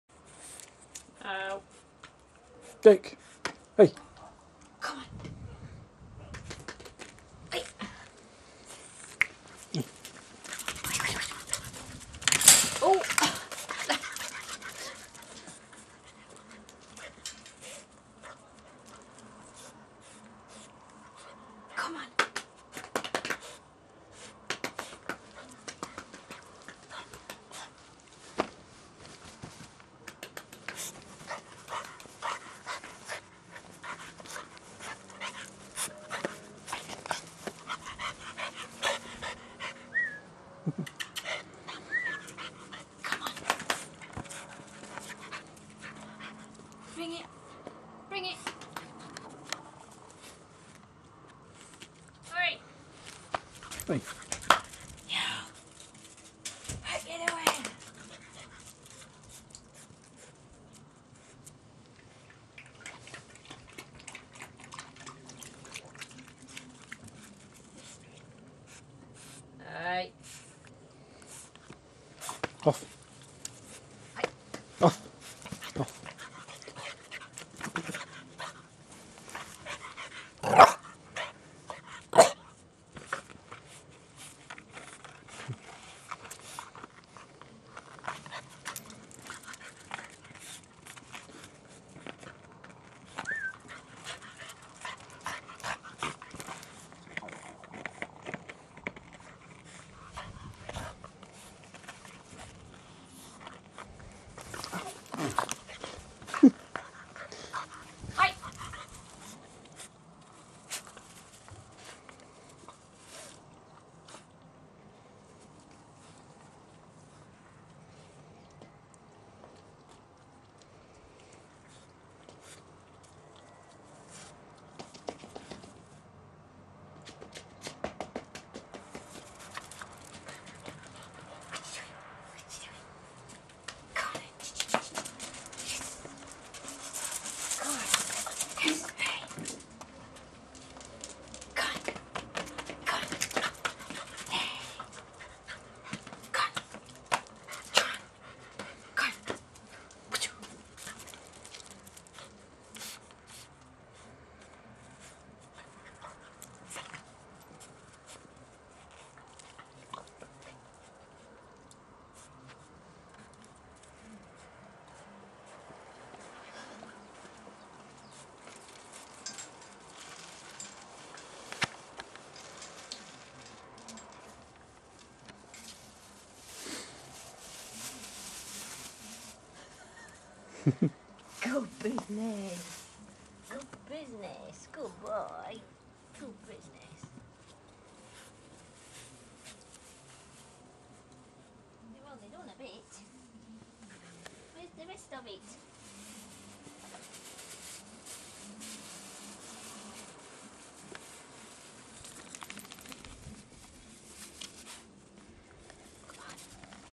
staffi puppy playing with ball
12 week old staffi playing with ball at night .